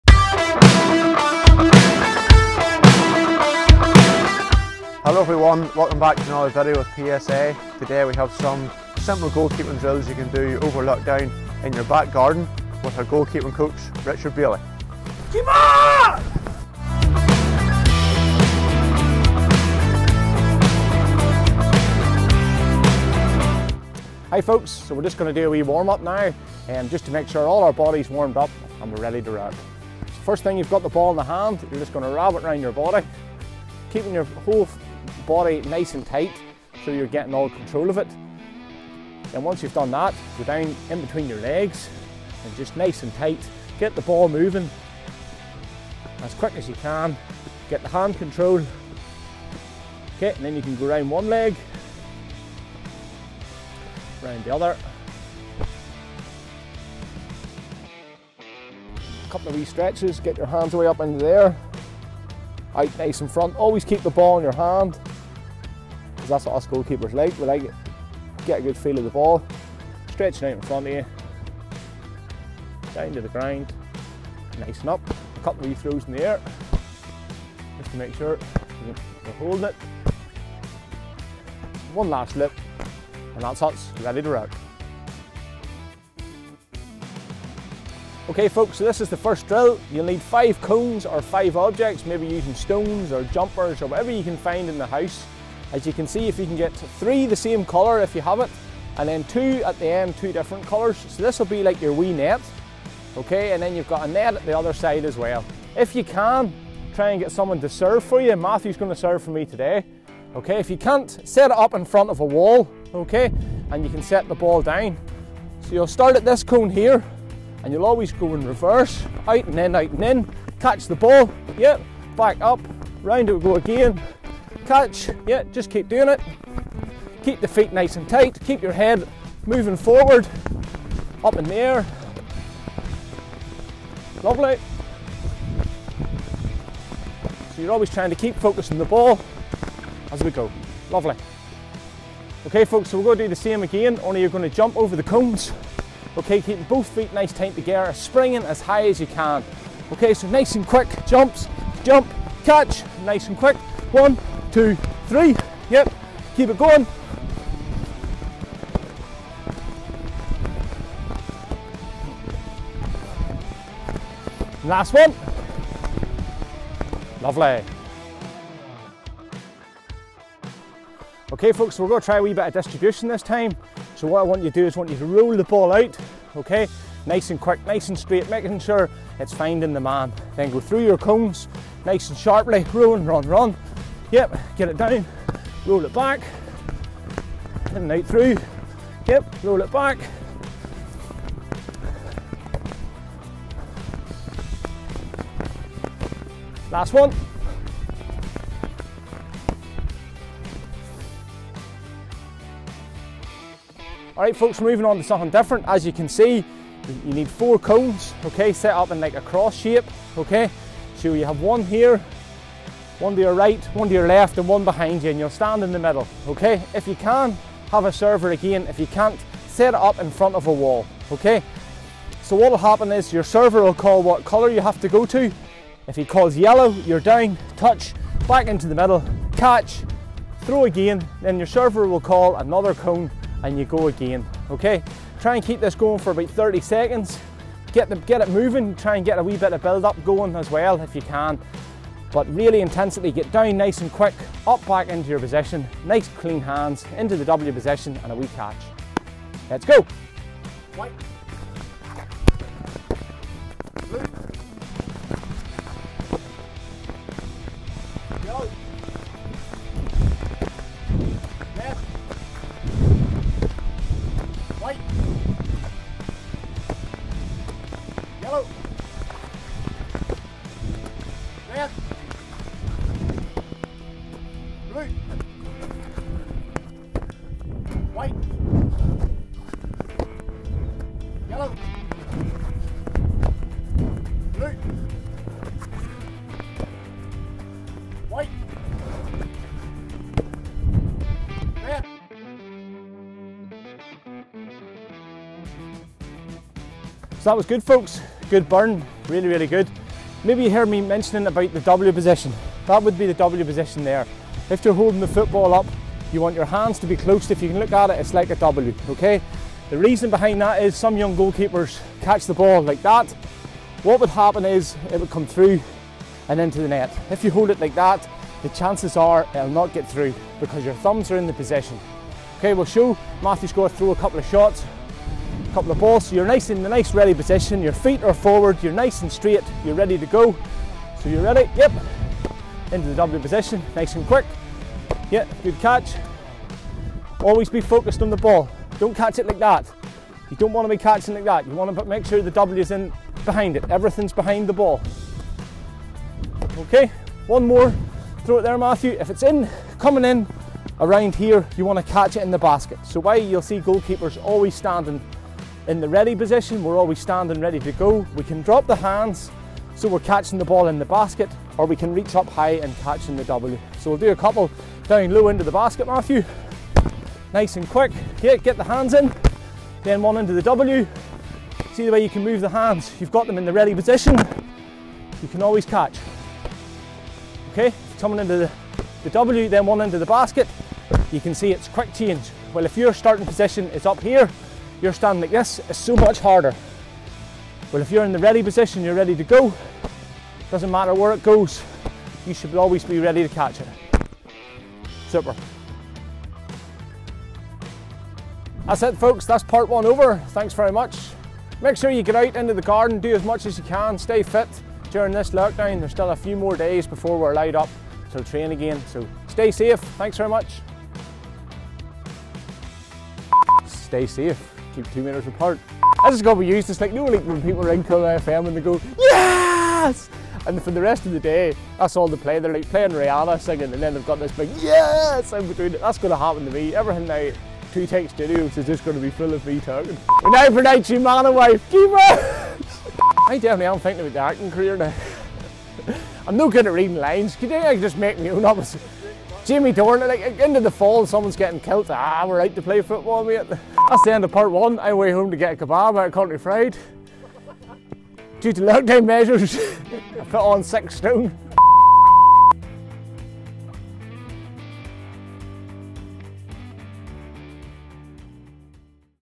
GOALKEEPER DRILLS
This week, our goalkeeping coach Richard takes us through some warmups and goalkeeping drills!